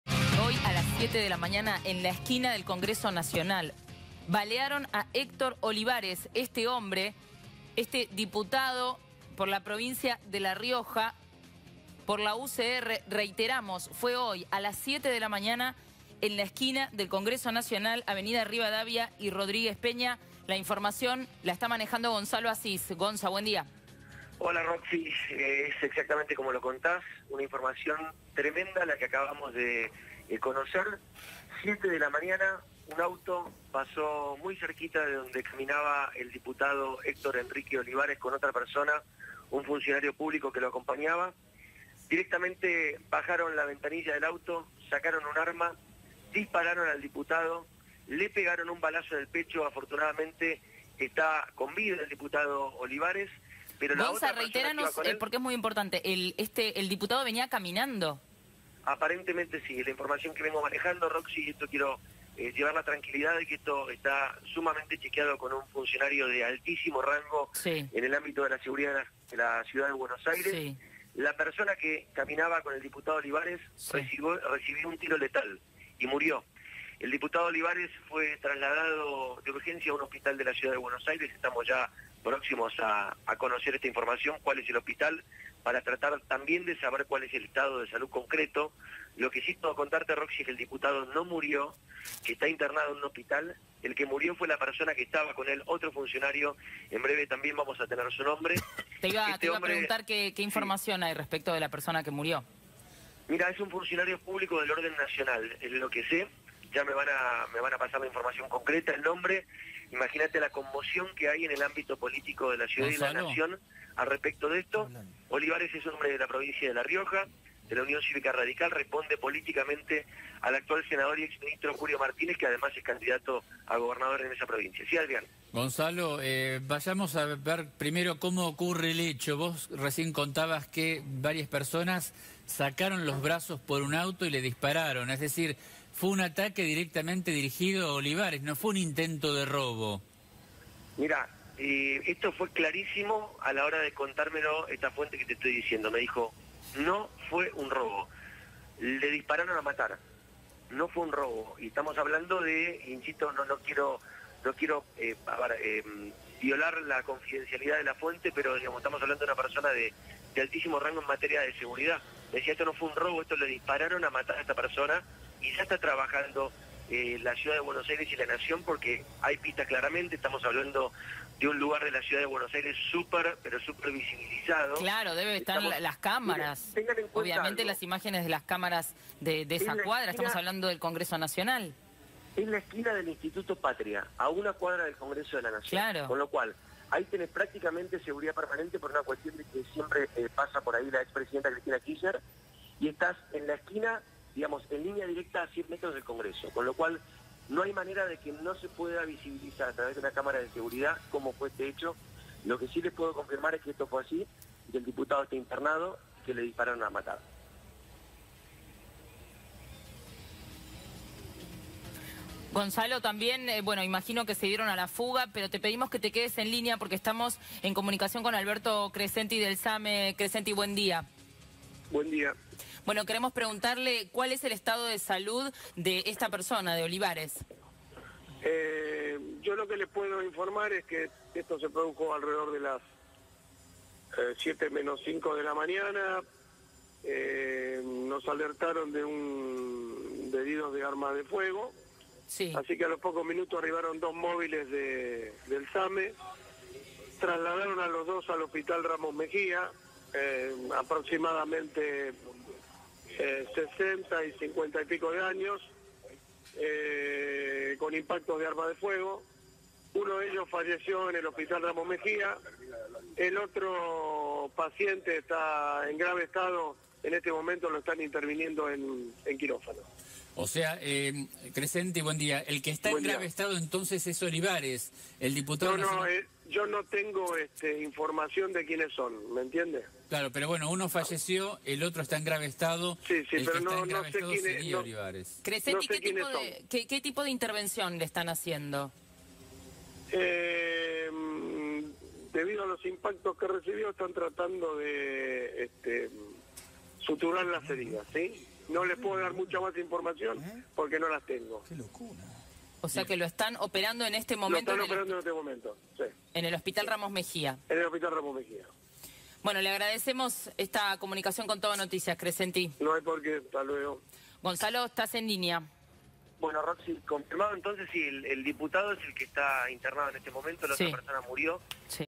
Ataque a un diputado en la esquina del Congreso: balearon al diputado Héctor Olivares, de Cambiemos
Las primeras hipótesis descartan que el ataque a balazos al diputado Olivares haya sido en el marco de un asalto en la esquina del Congreso: fue un ataque desde un auto en movimiento.
Hay dudas y muchas sospechas.
Uno de sus colaborares murió y el diputado lo están operando.